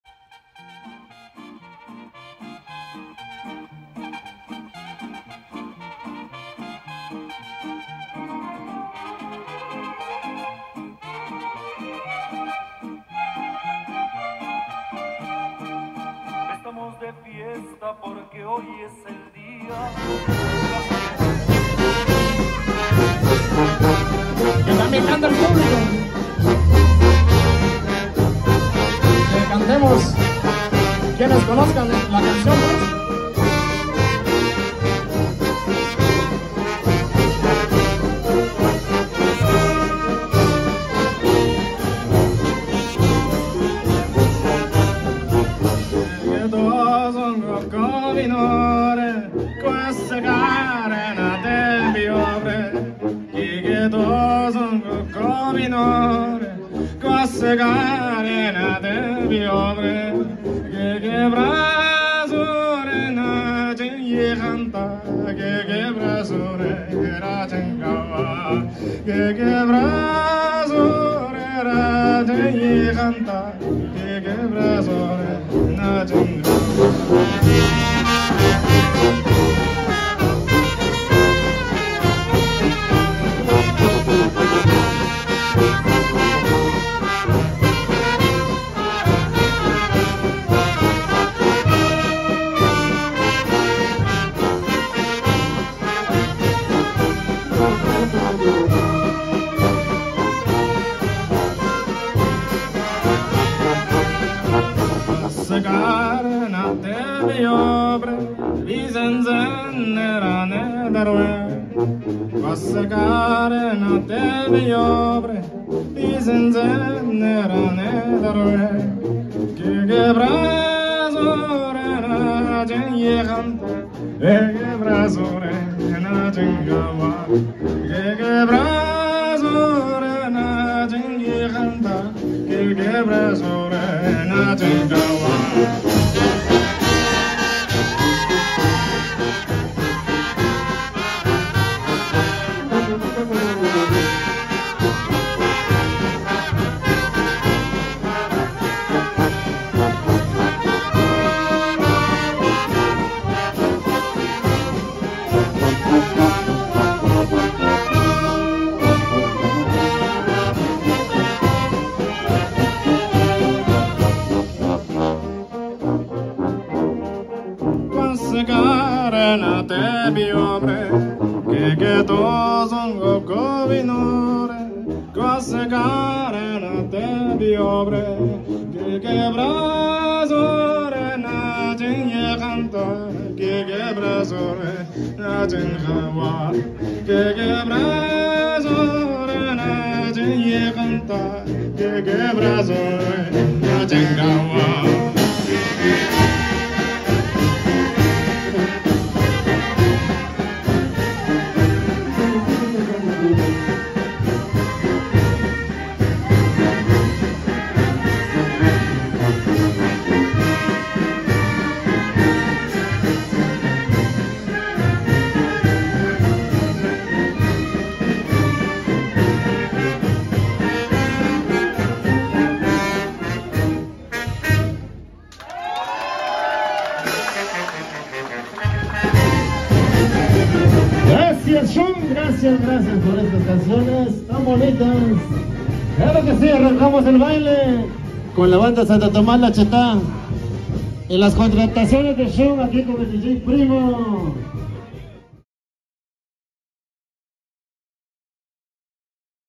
El Cantante japonés en las festividades de San Andres Solaga 2023
La Banda Juvenil San Andres Solaga radicados en Los Angeles California invitaron al publico en general a su gran fiesta en honor a su santo patron la Virgen del Carmen  el Domingo 9 de Julio del 2023.